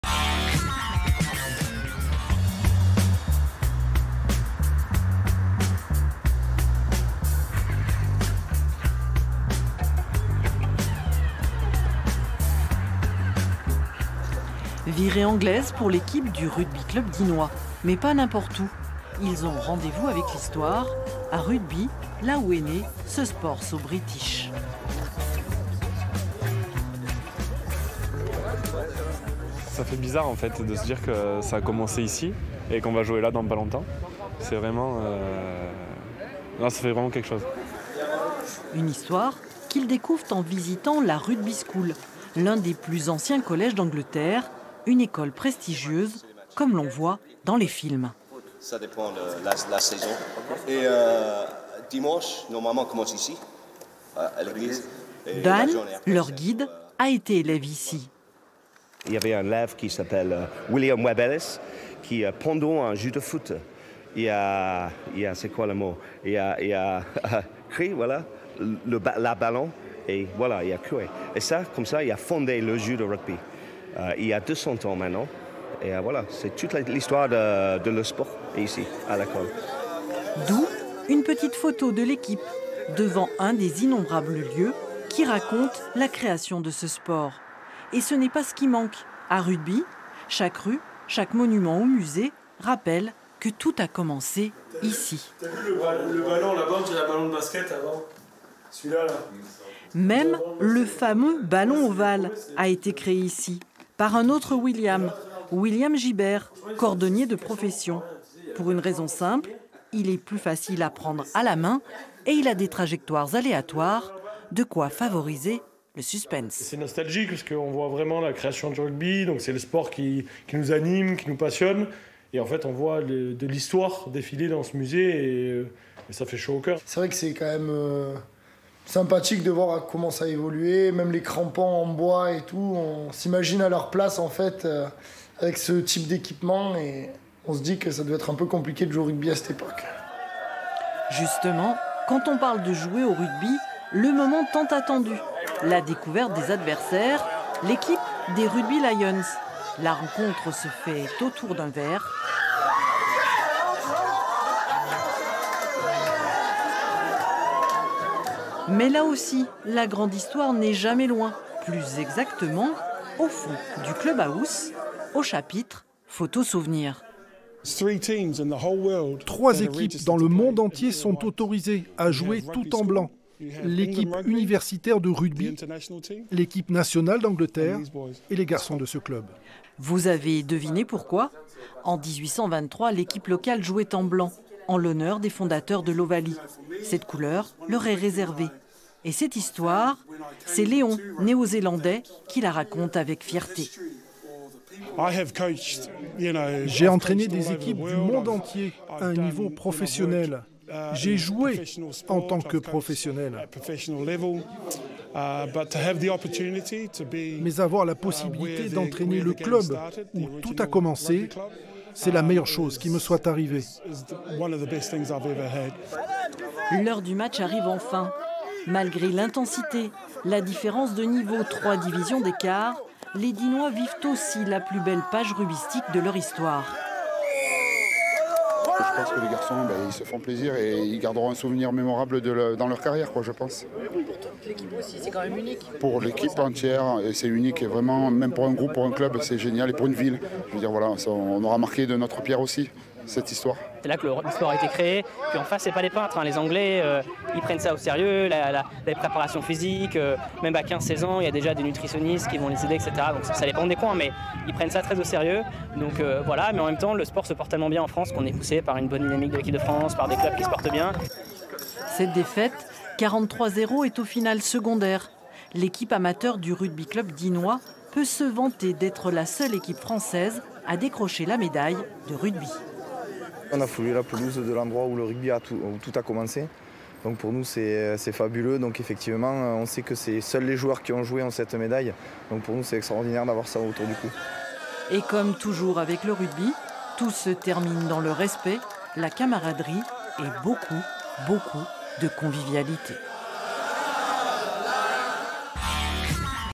Le club de rugby de Digne-les-Bains invité à célébrer la naissance de ce sport en Angleterre
Direction l’Angleterre, où on fête un bicentenaire : celui de la naissance du rugby dans la ville du même nom. C’était en 1823. Seuls invités étrangers pour ces célébrations : l’équipe de Digne-les-Bains, organisatrice du mondial amateur de rugby.

Reportage de : Véronique Bouvier, Aurélien Casanova, Bruno Carapic